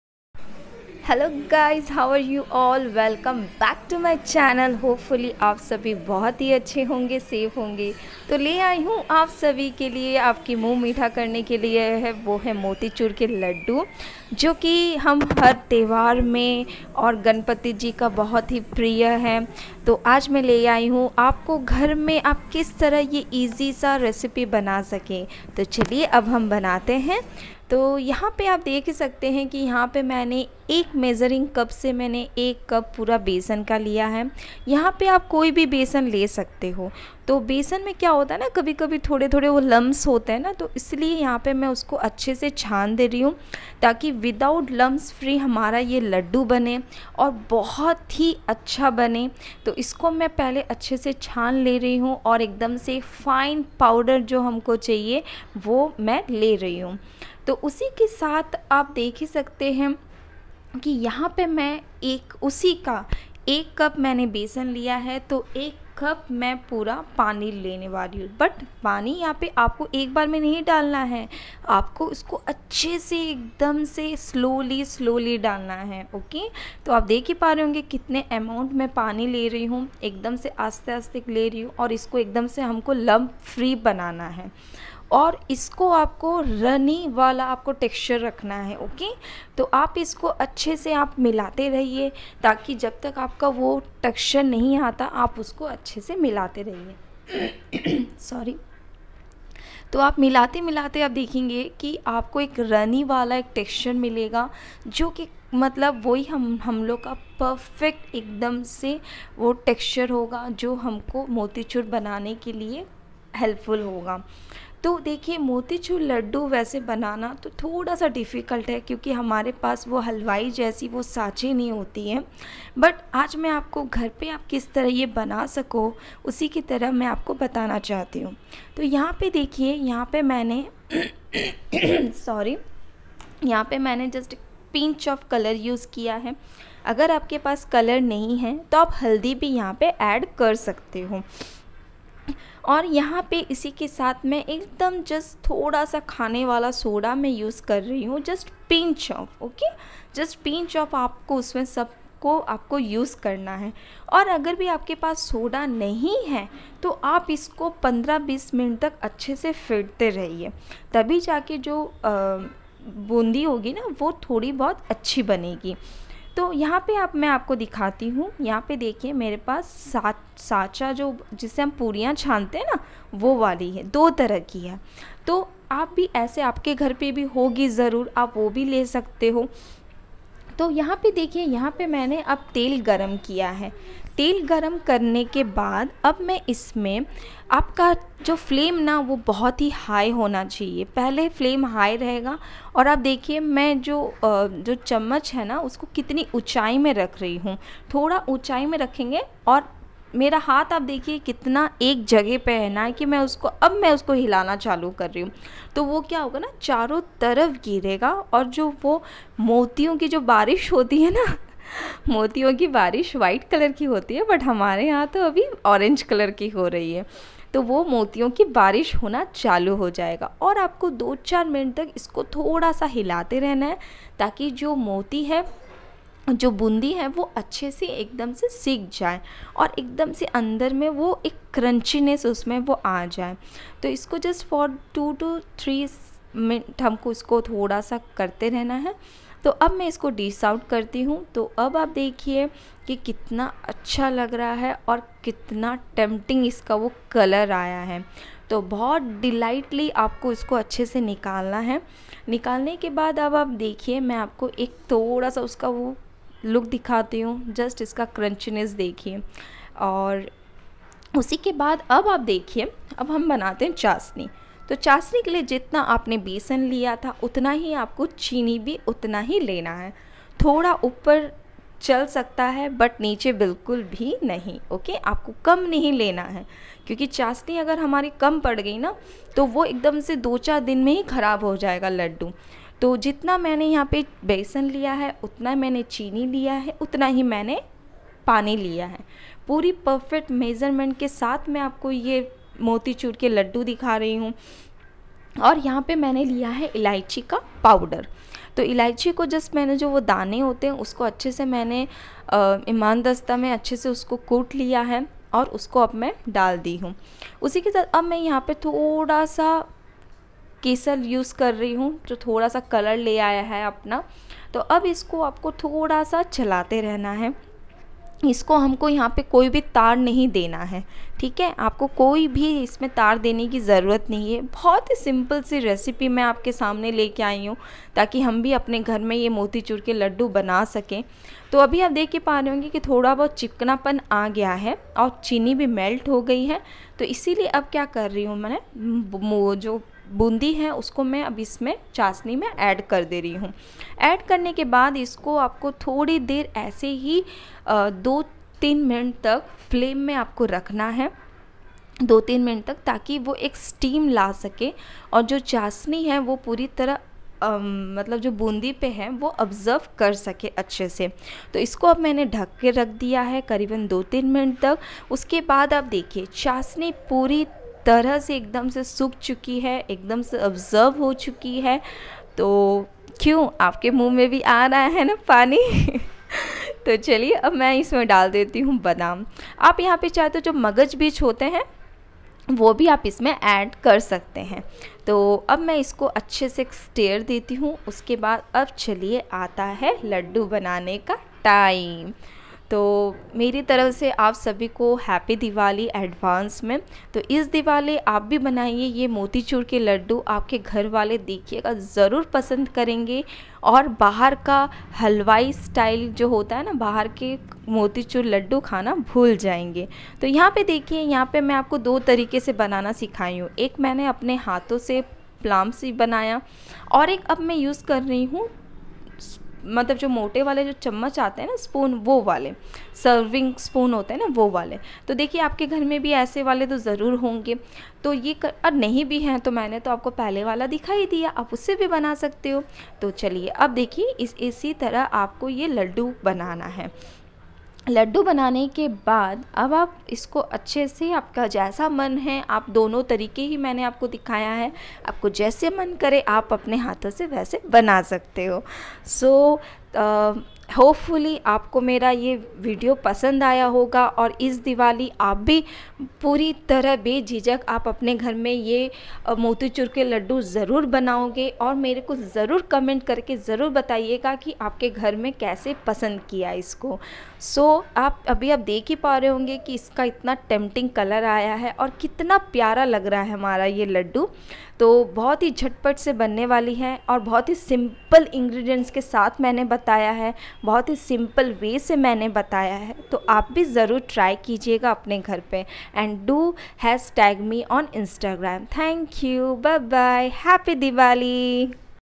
बिना झारे के हलवाई जैसी मोतीचूर के लड्डु नये तरीक़े से - Motichoor laddu Recipe Diwali Sweet.
Boondi ladoo without jhara, bina jhara  ke,motichur ladoo ,festival sweet recipe ,diwali ki mithai, boondi k laduu kaise banaye,motichur ladoo recipe